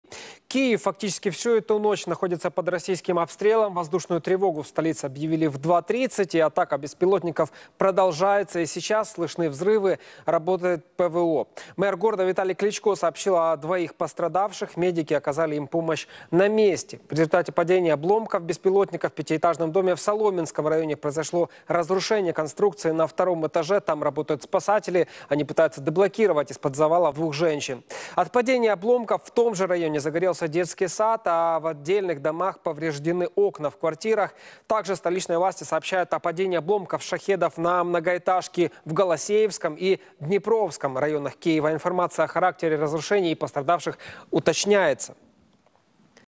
⚡️ Ночная АТАКА по Киеву! ПОСЛЕДСТВИЯ НАПАДЕНИЯ БПЛА РФ
В ночь на 25 ноября Россия выпустила рекордное количество дронов Shahed-131/136 – около 75. Основное направление удара было на Киев.

В результате воздушной атаки на столицу пострадали пять человек, среди них 11-летний ребенок.

В Соломенском районе возгорание в нескольких местах – в жилом доме и нежилых помещениях.